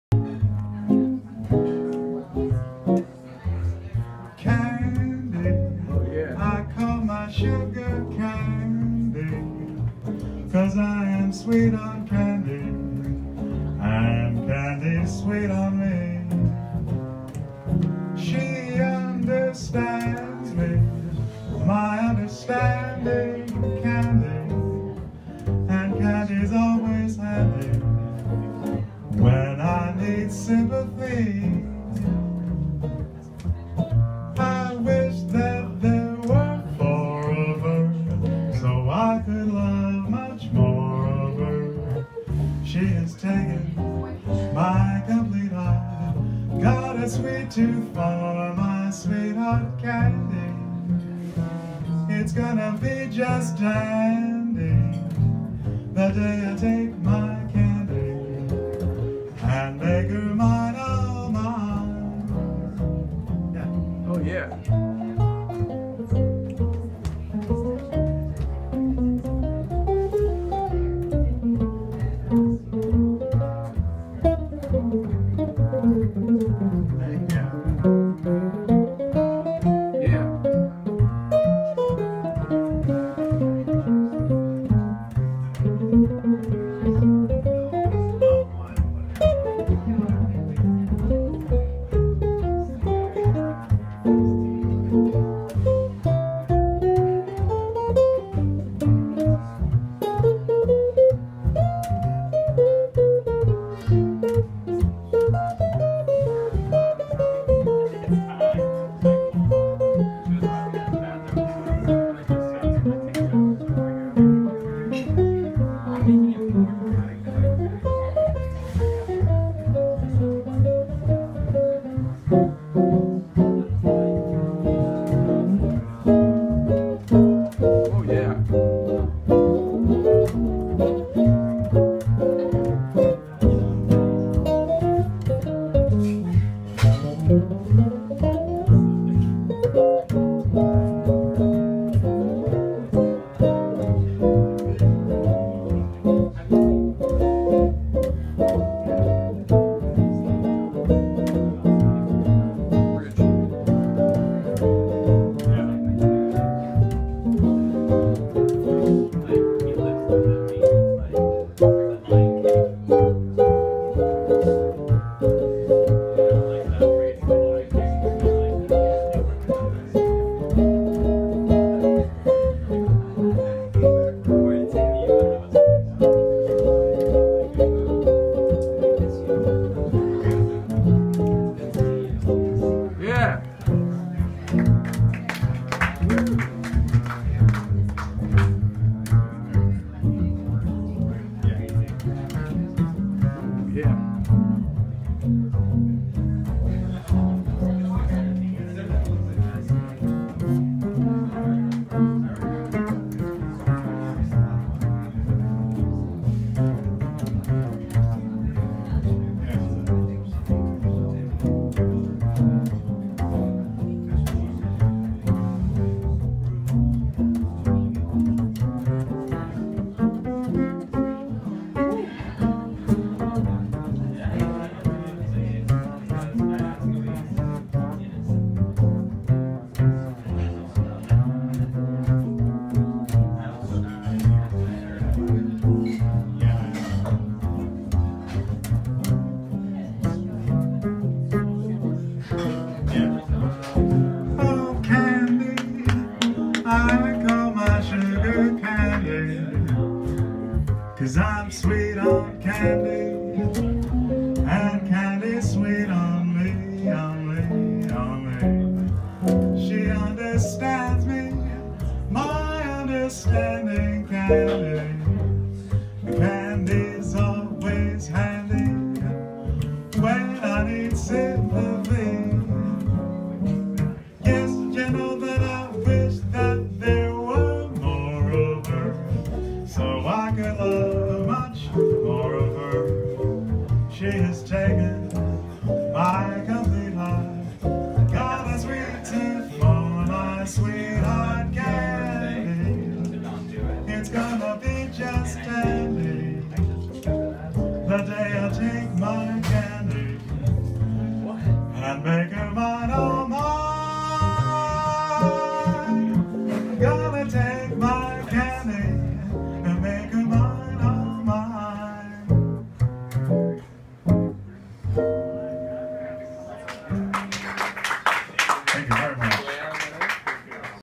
Stephen Santoro- Ron McClure
Stephen Santoro- Vocal
Ron McClure - Bass
Corey Larson - Guitar
Swing 46 - NYC - 7/5/18